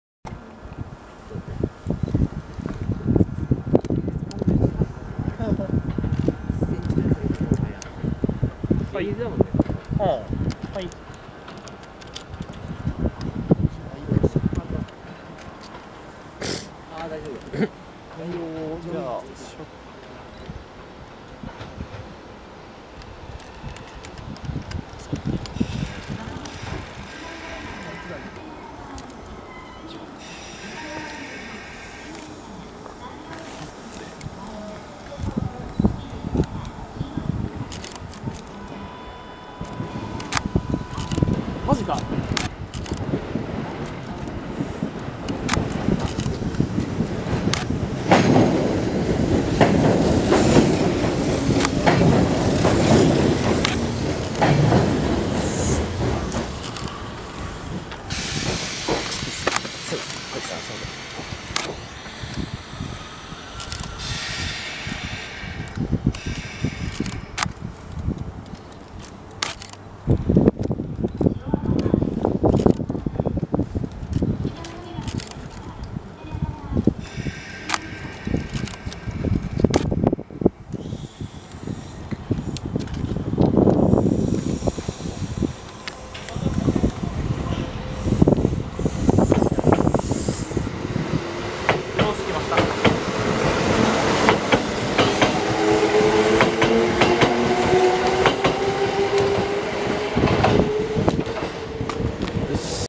近鉄UB03+AS25 五位堂出場回送 塩浜にて
2019/12/23撮影
この日は有給を取っていたので、夜も塩浜Bに参戦して来ました〜1時間前に到着しましたが既にキャパが埋まっており、激パでしたが、身内の方がいたので入らせて頂きました。ありがとうございますw